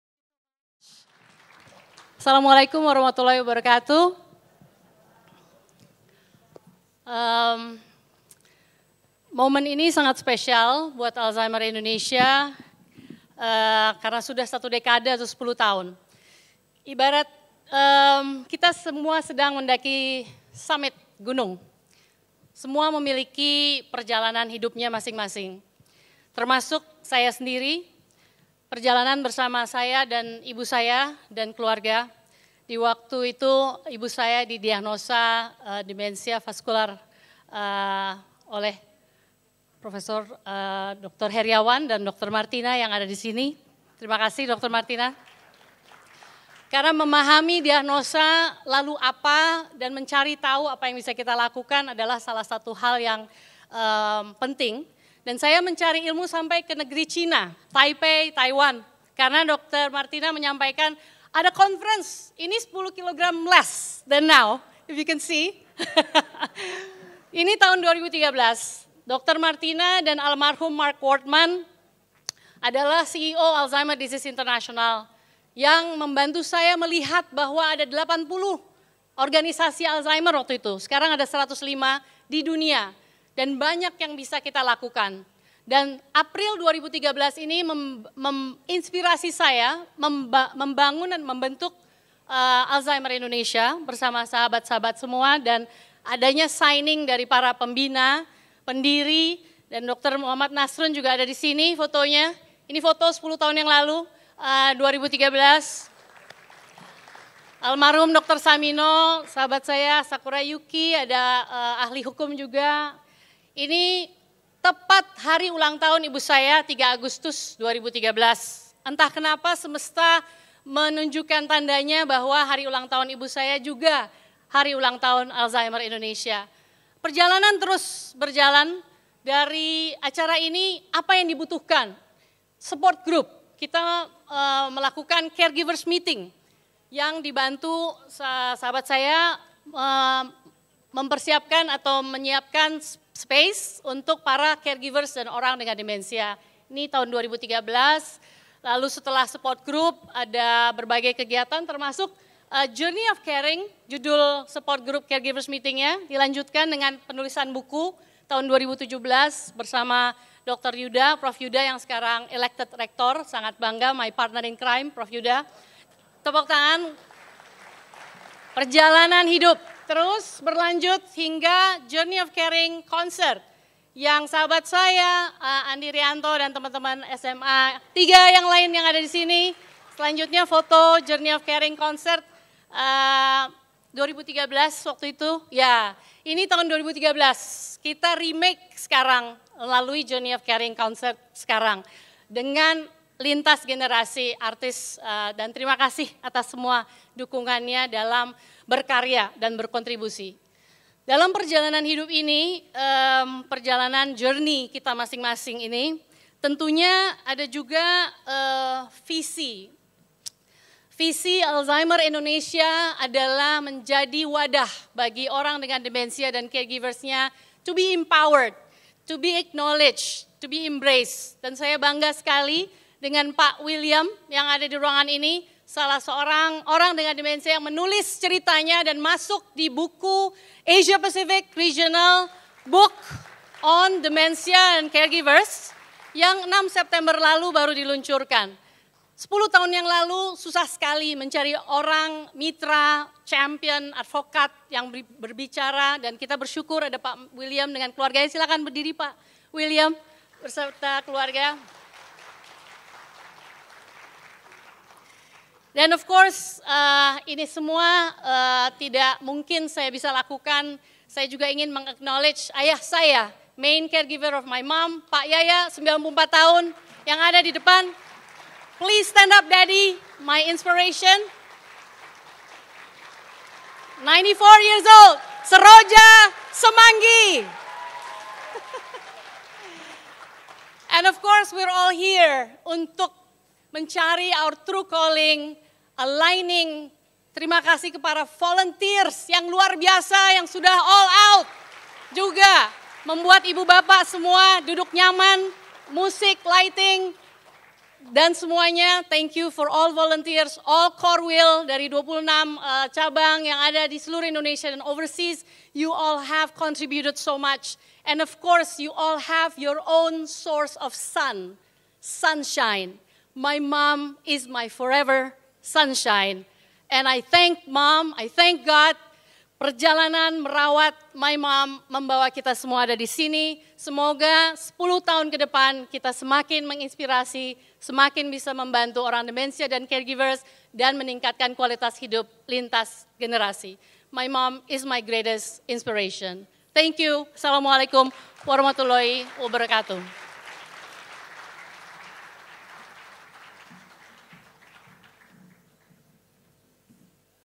DY Suharya - Sambutan pada "Journey of Caring" Charity Concert
Memperingati 10 Tahun (Satu Dekade) Bersama ALZI & Hari Alzheimer Sedunia, Alzheimer's Indonesia & Partners mempersembahkan Journey of Caring Charity Concert yang menampilkan Andi Rianto, Keisya Levronka, Kevin Widaya, Dee Lestari, Farman Purnama, Harvey Malaihollo, Erwin Parengkuan, Shinta Priwit, Yanti Djani, dengan Special Guest Star: Widyawati.

Sabtu, 23 Sep 2023, 15:00 - 17:00 WIB
Unika Atma Jaya, Gedung Yustinus Lt. 15, Jl. Jendral Sudirman No. 51, Jakarta Selatan